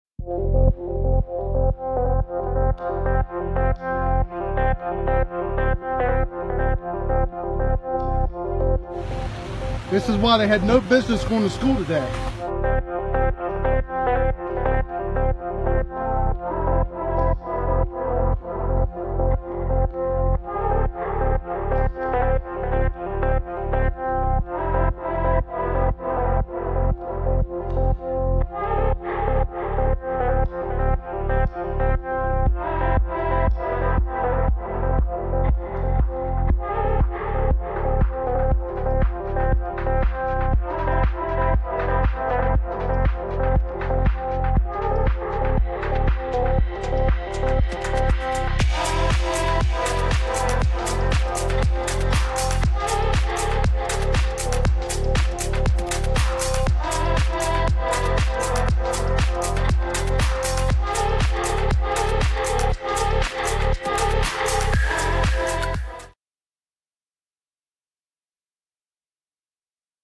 School bus slides off road and into creek during snow
A school bus slid off the road and into a creek in Maryland, where there have been hundreds of crashes during the fourth nor'easter in three weeks -- or as some are calling it, the "four'easter."